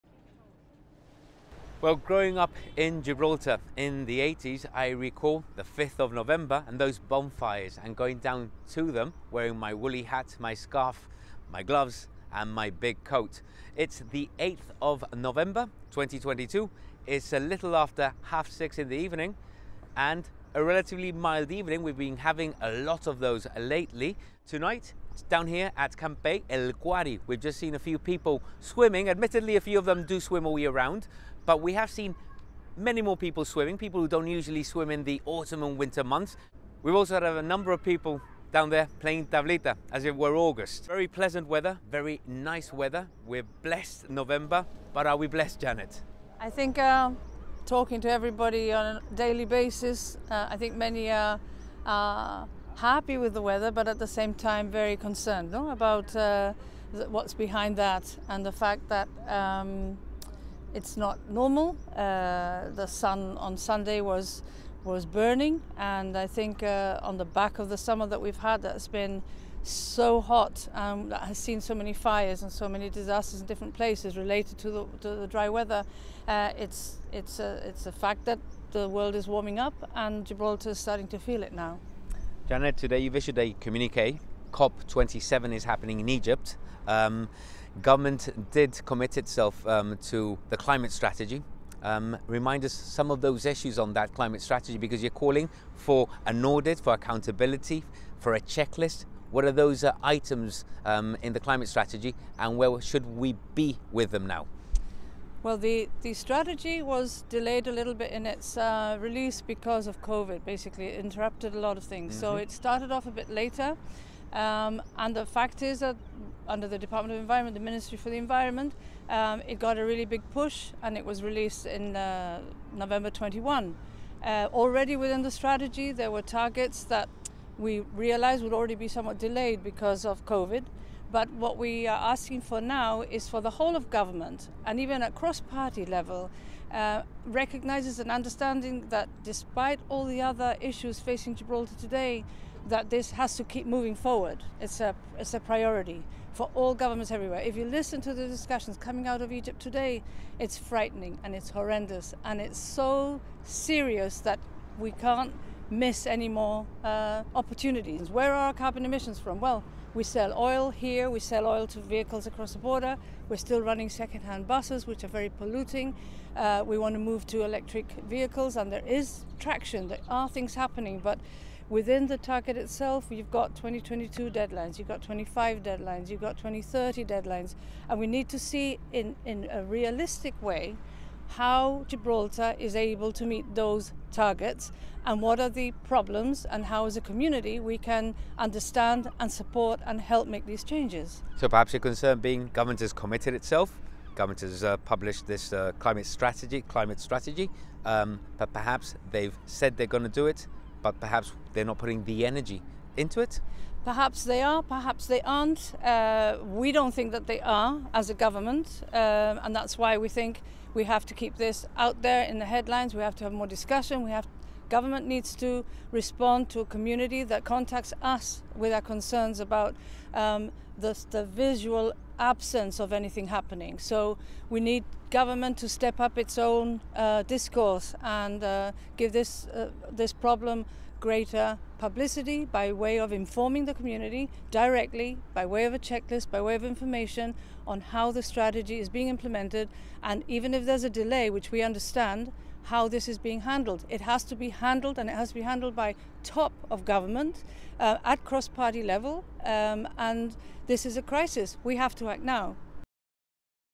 The Environmental Safety Group calls for renewed focus on Gibraltar's climate strategy targets
The Environmental Safety Group says the community should be kept informed of the progress in meeting Gibraltar’s Climate Strategy Targets and how the Climate Change Committee is helping to drive these goals.

The pressure group hopes Gibraltar will embrace the opportunities for a green revolution and the application of best available technology.

As the sun went down, our reporter Kevin Ruiz spoke to the ESG's Janet Howitt.